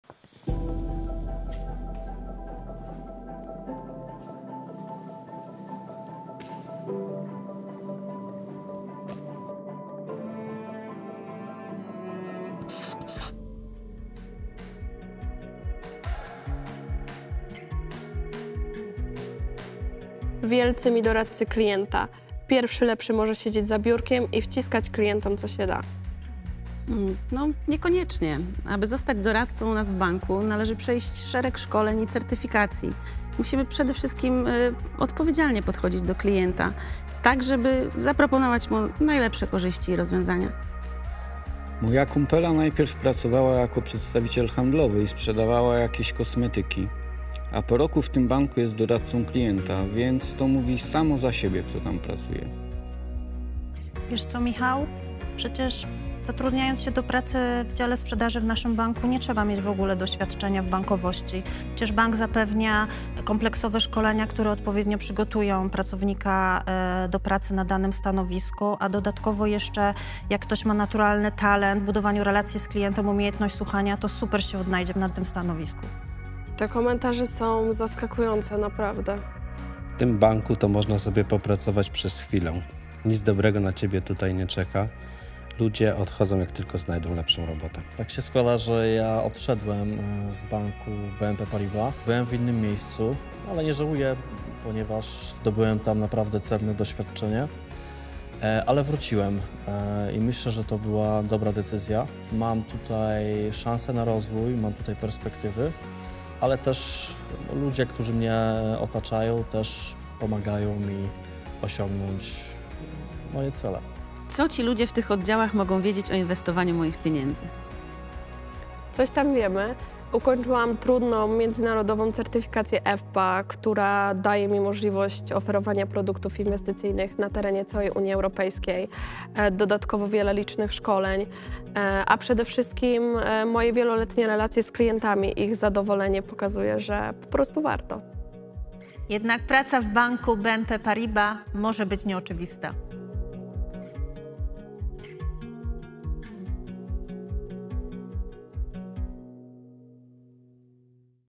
Obszar kompetencji i możliwości rozwoju - sprawdzamy, jak jest
Skonfrontowaliśmy anonimowe komentarze i opinie znalezione w Internecie na temat postrzegania pracy w sieci sprzedaży z rzeczywistymi doświadczeniami pracowników i pracowniczek, pracujących na co dzień w Centrach Klienta Banku BNP Paribas.

Odwiedź stronę i dołącz do nas. Dobre decyzje bywają nieoczywiste.

W filmie udział wzięli:
Julia, doradczyni Klienta premium @stylistka innowacyjnych rozwiązań
Karolina, starsza doradczyni Klienta biznesowego @badaczka biznesowych potrzeb
Ewa, ekspertka ds. Klienta premium @ambasadorka wartościowych relacji
Michał, starszy doradca Klienta biznesowego @twórca niezawodnych strategii
Dariusz, dyrektor centrum doradczego bankowości detalicznej i biznesowej @mistrz bankowych więzi
Damian, starszy doradca Klienta premium @kreator najlepszych rozwiązań